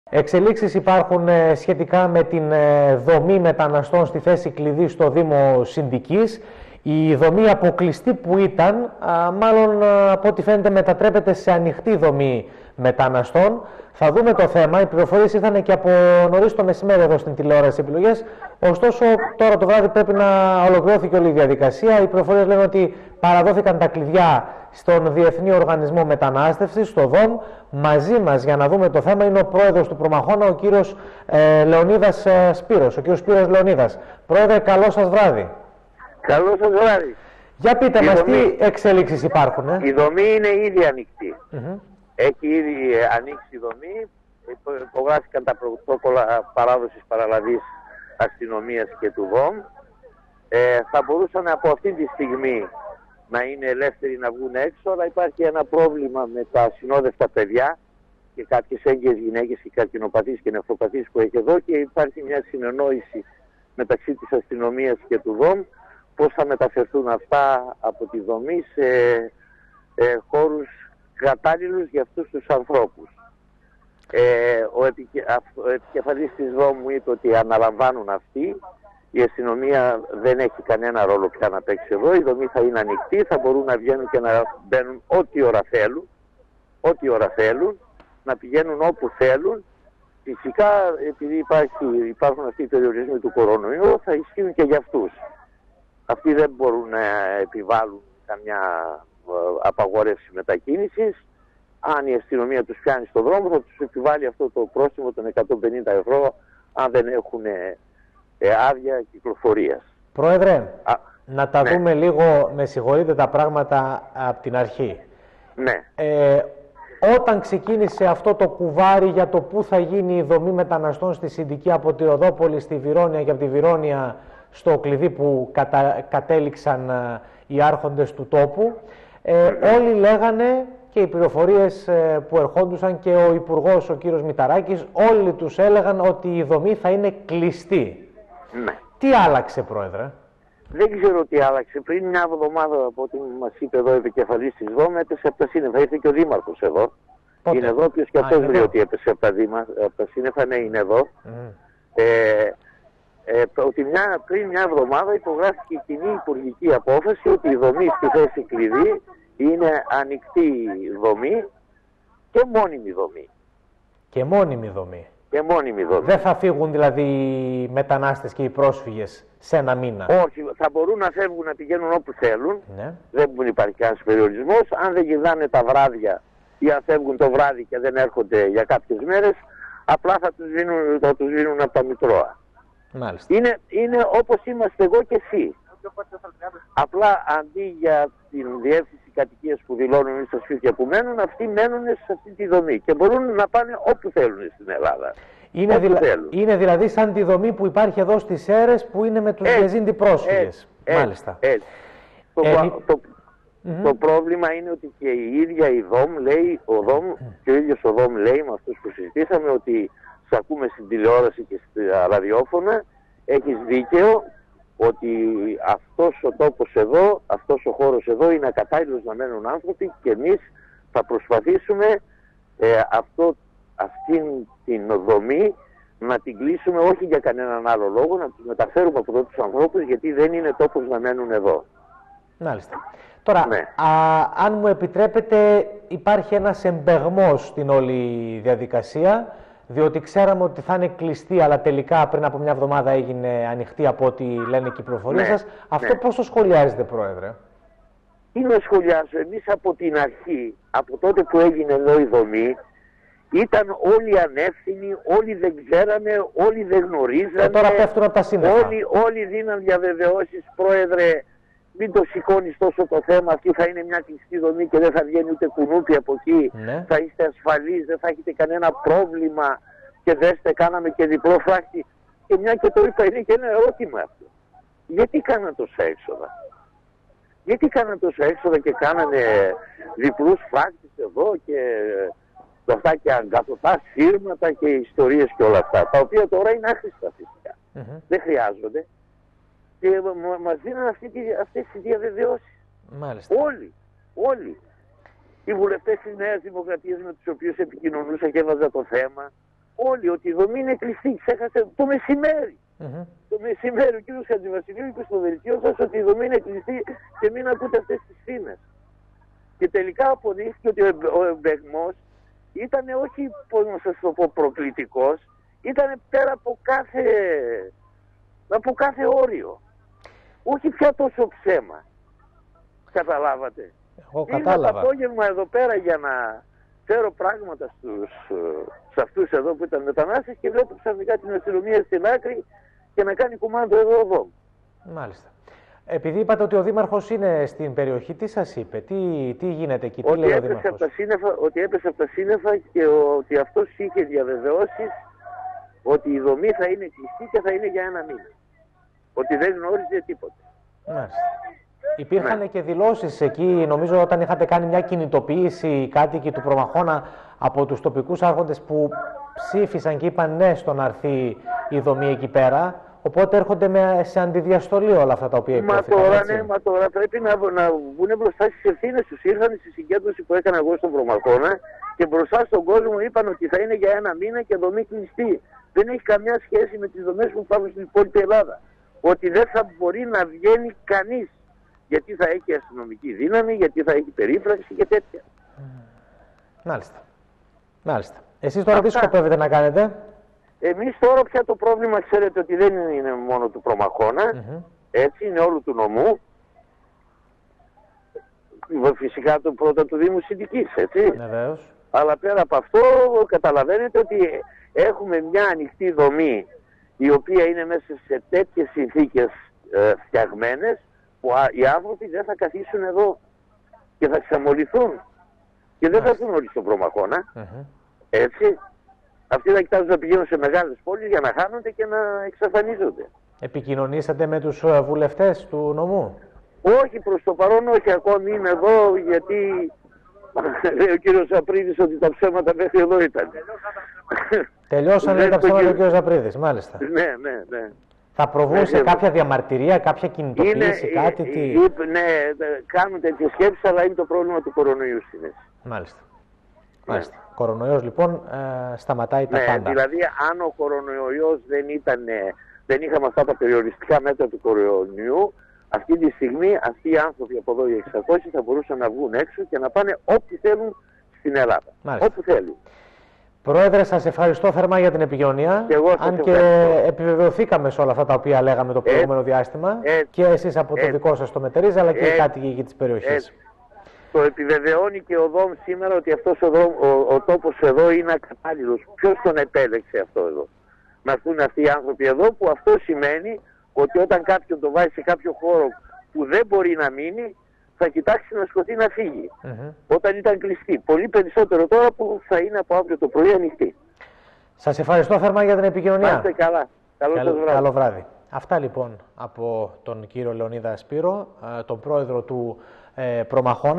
Σέρρες: Ανοιχτή πλέον η δομή μεταναστών στο Κλειδί
Σε ανοιχτή δομή μετατράπηκε πλέον η δομή στο Κλειδί Σερρών, όπως δηλώνει στο Επιλογές ο πρόεδρος του Προμαχώνα κος Σπύρος Λεωνίδας